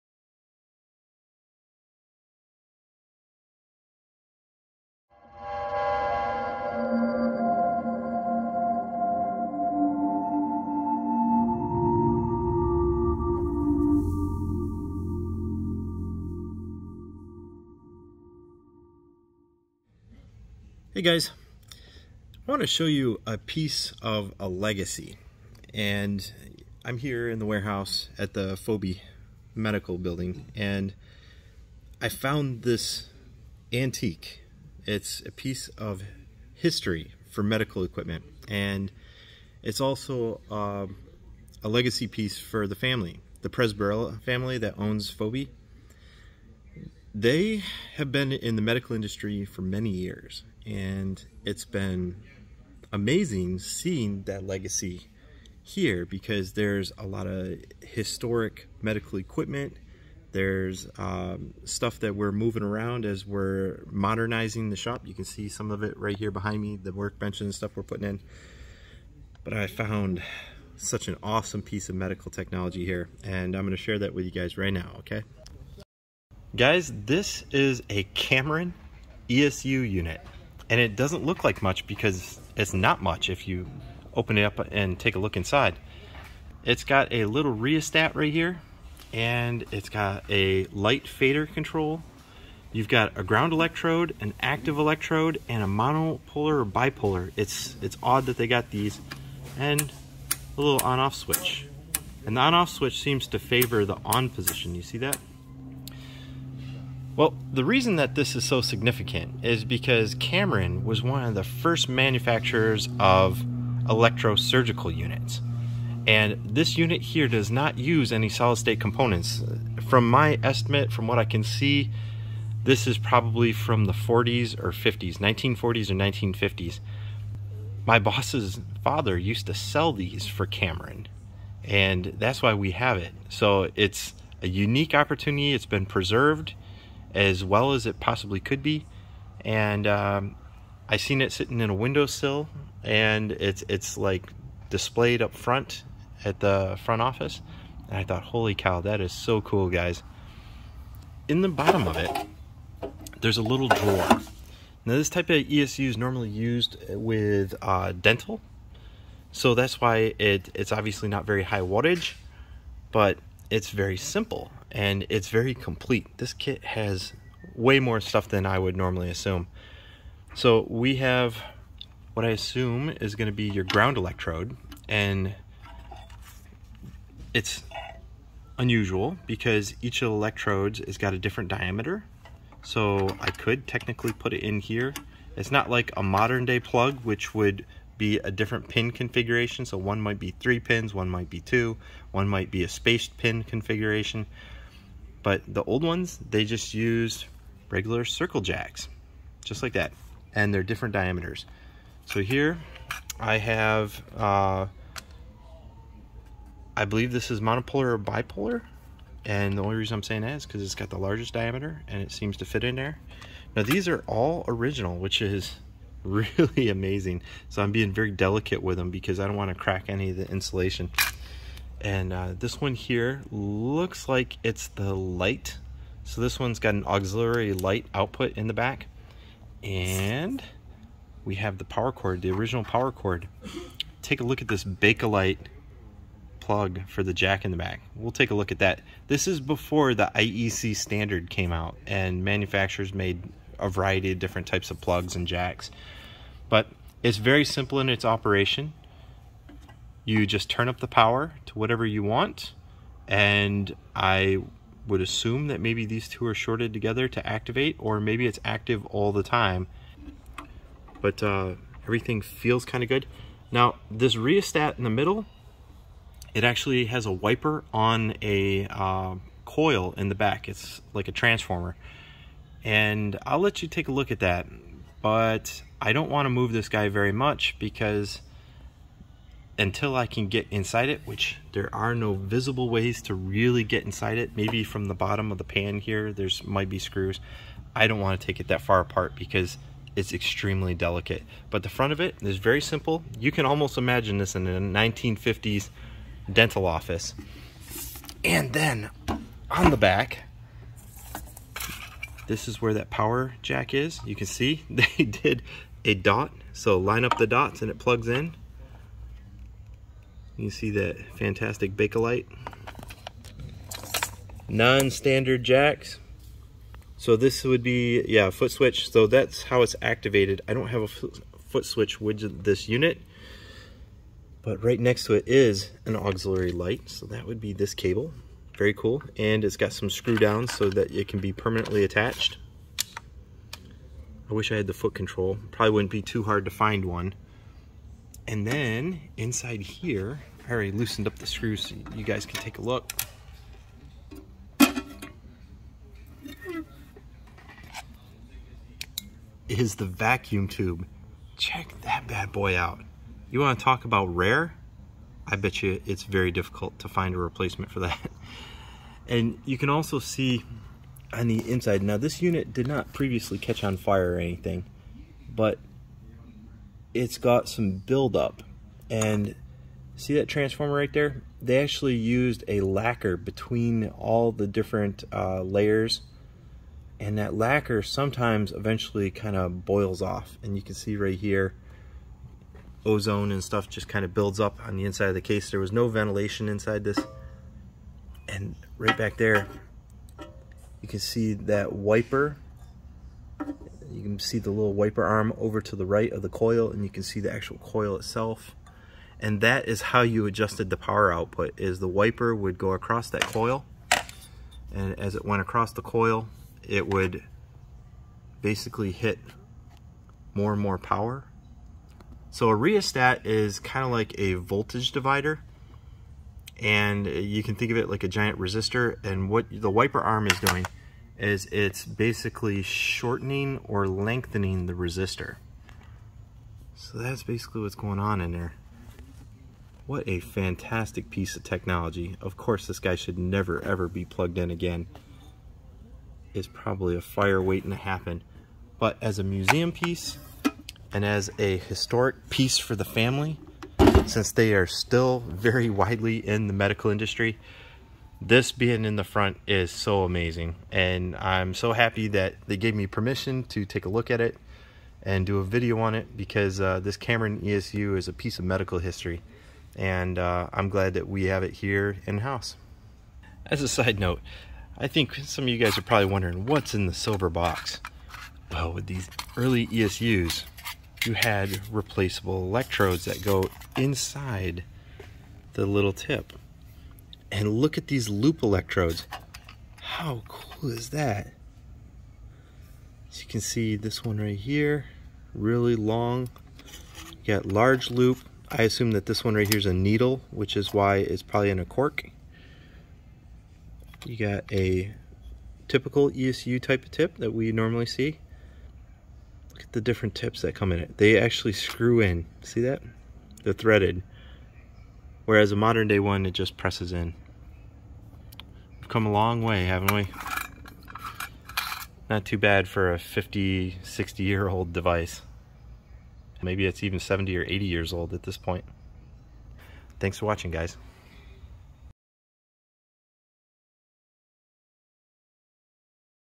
A Piece of Medical History: Historic ESU
The Cameron Generator is one of the first ESUs for the commercial market.
Cameron later joined the Miller company and produced Cameron Miller ESUs. I am trying to grow this channel to deliver better content.